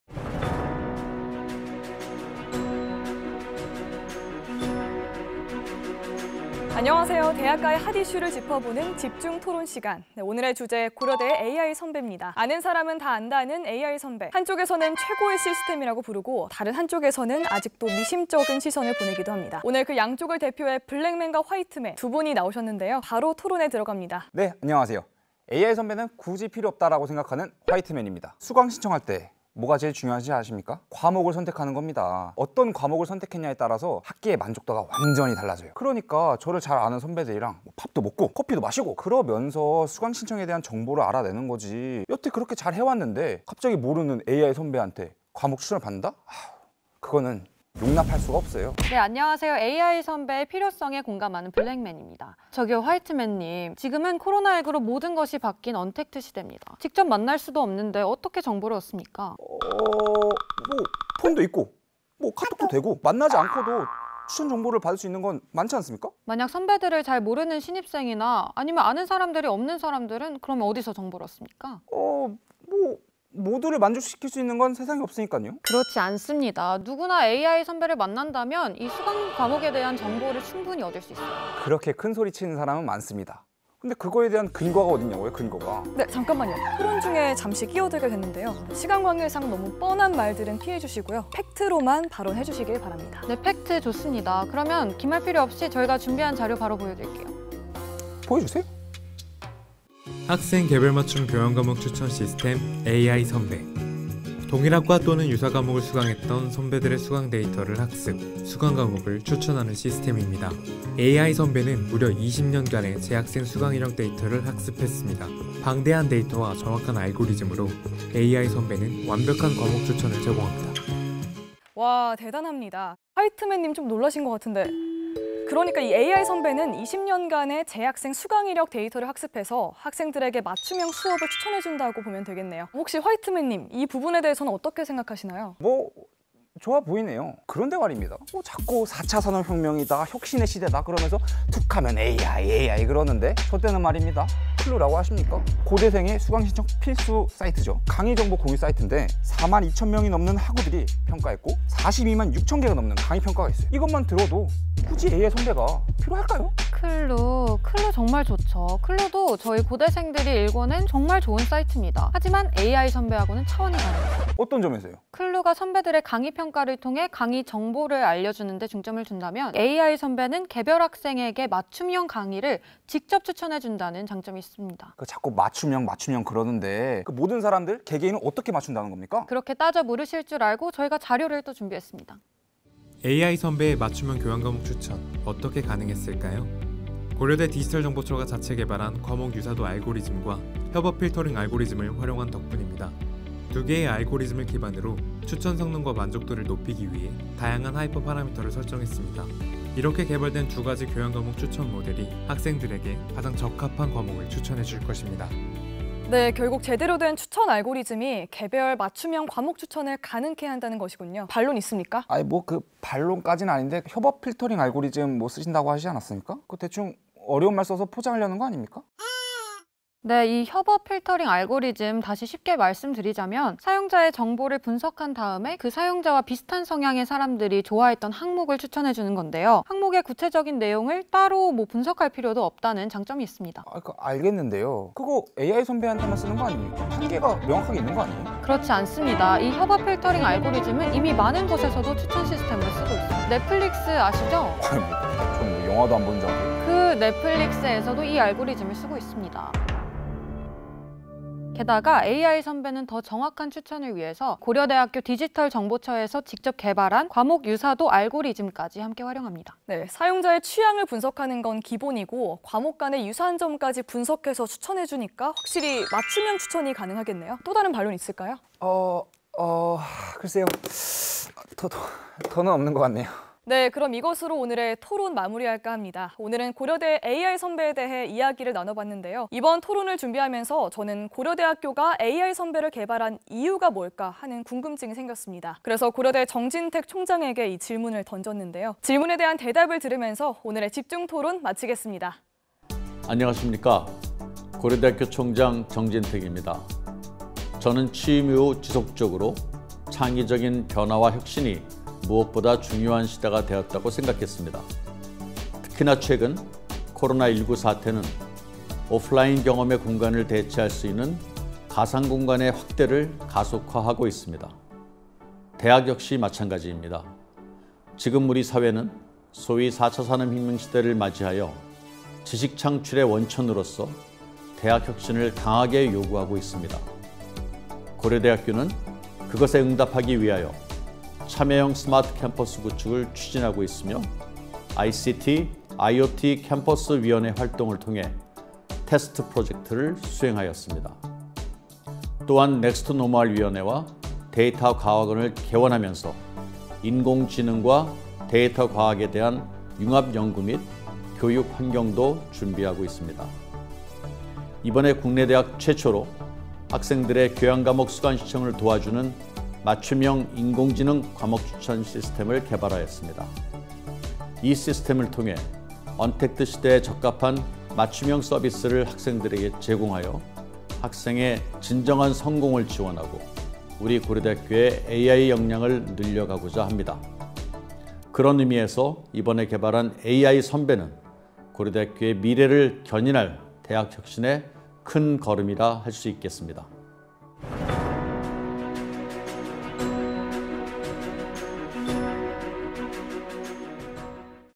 [고려대학교 Korea University] 디지털정보처 AI선배 홍보 영상
학생들에게 맞춤형 교양 과목을 추천해주는
디지털정보처 AI선배 서비스 홍보 영상